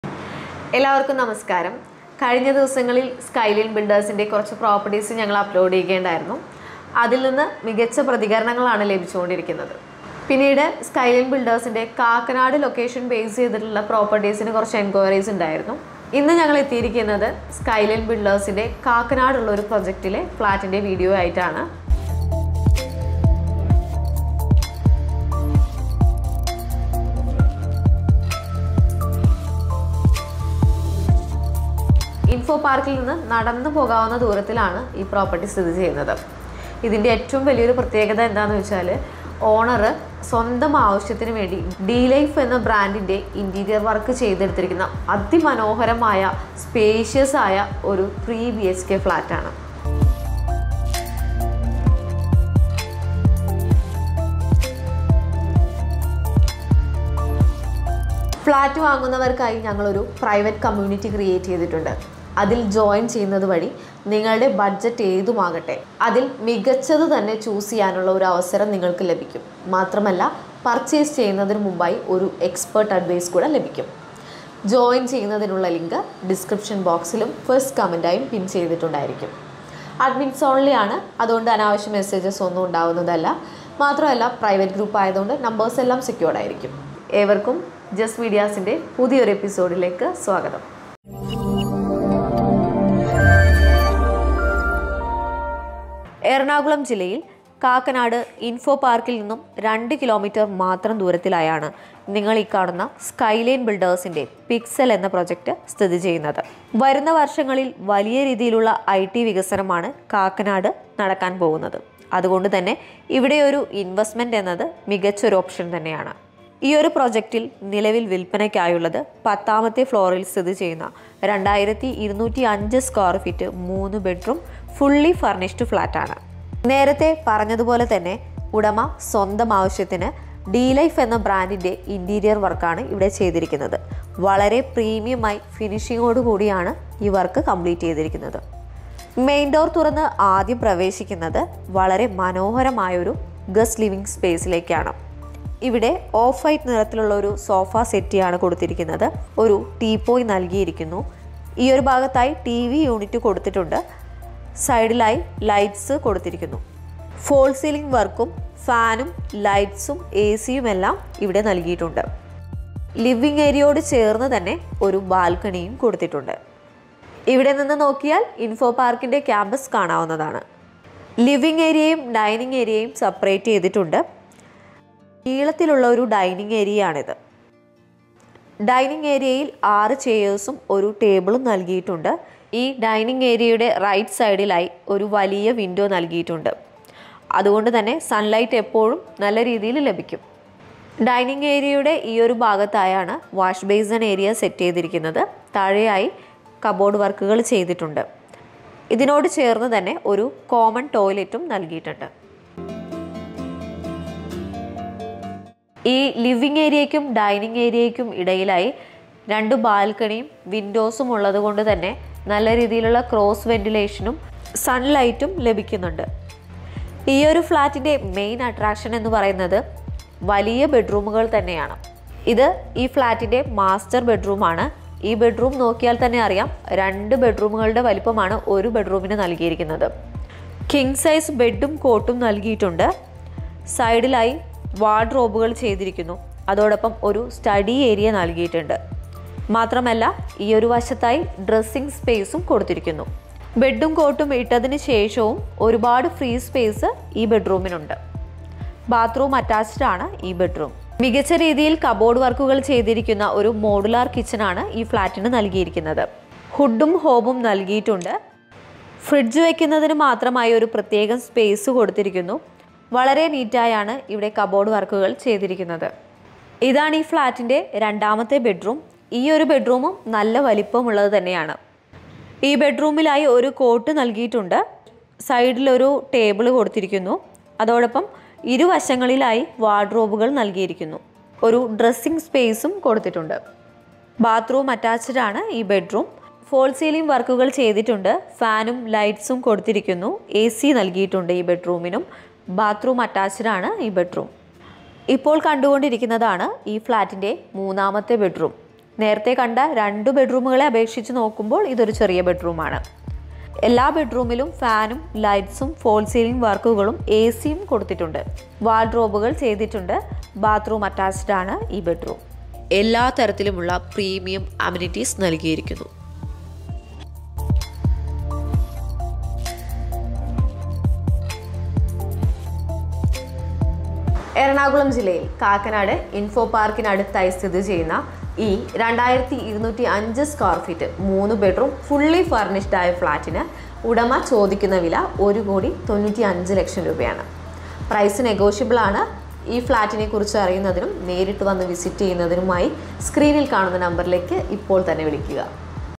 പുതിയ വലിയ ഫ്ലാറ്റ് | Skylines' 3BHK Furnished Flat Sale In Kakkanad Ernakulam | Skyline Builders
Property ID : FS290

Flat Sale In Kakkanad Kochi
Skyline Builders
Skyline Pixel
2205 Sqft 3 BHK
Asking Price 1.95 Cr Negotiable